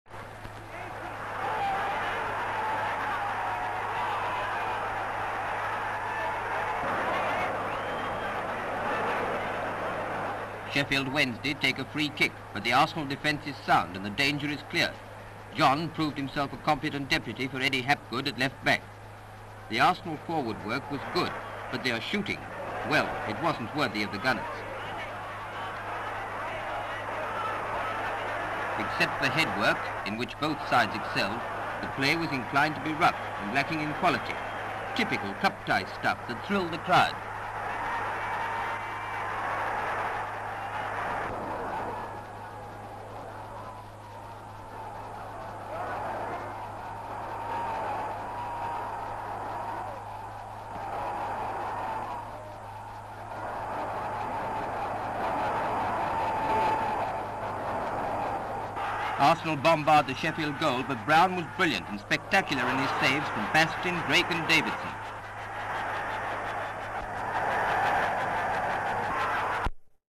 Extra! Sheffield Wednesday And Arsenal (1930-1939)
Title reads: "Extra! In view of its especial interest Pathe Gazette includes additional pictures of Sheffield Wednesday V Arsenal".

Various shots covering football match between Sheffield Wednesday and Arsenal. Bastin is playing for Arsenal. Brown was in goal for Sheffield.

N.B. Item found in Unidentified Gazettes reel. Possibly from the 1935 Football Association FA Cup sixth round.
 FILM ID:2955.1